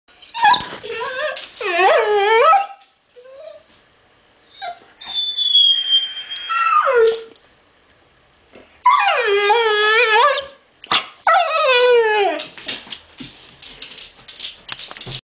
Pit bull talking.
Cali my 11 month old pitbull looking for attention and she dose it by whining LOL.